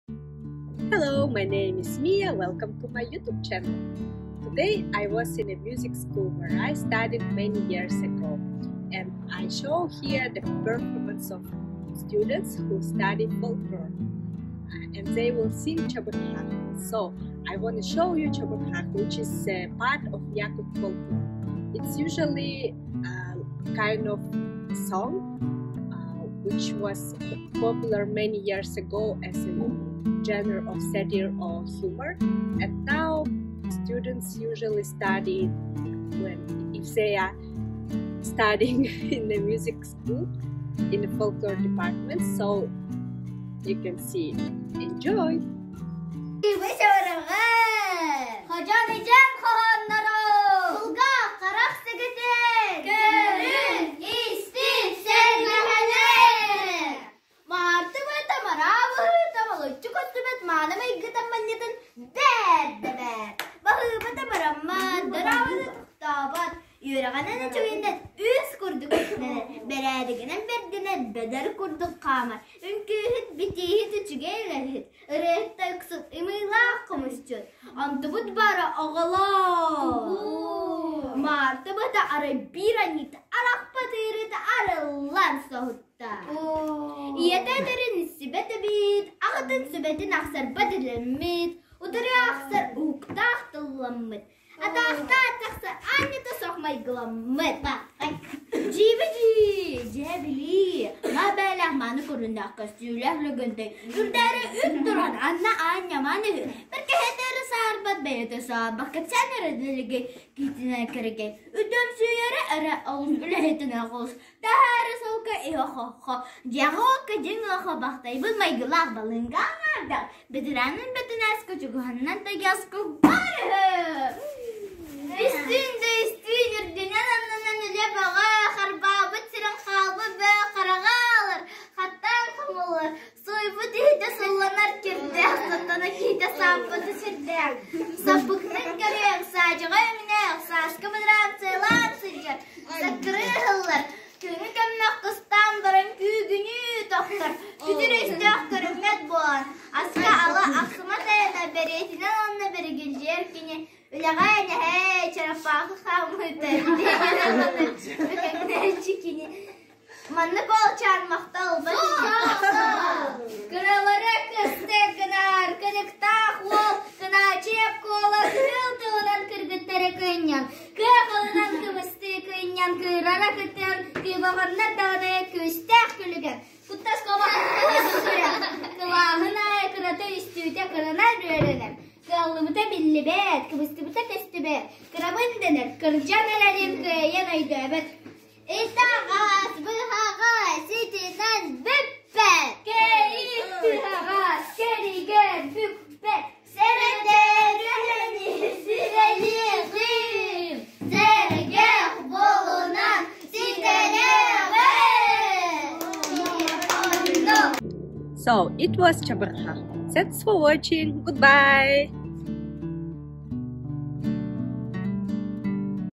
Yakut folklore | Chabyrgakh
In this video you can see chabyrgakh.

Chabyrgakh is a genre of folklore of the Yakut people which was popular as a genre of satire and humor in the past. Nowadays, it has particular success in amateur and professional art. Students study this kind of art at the music school in the department of folklore.

Please, listen, enjoy and write your opinion!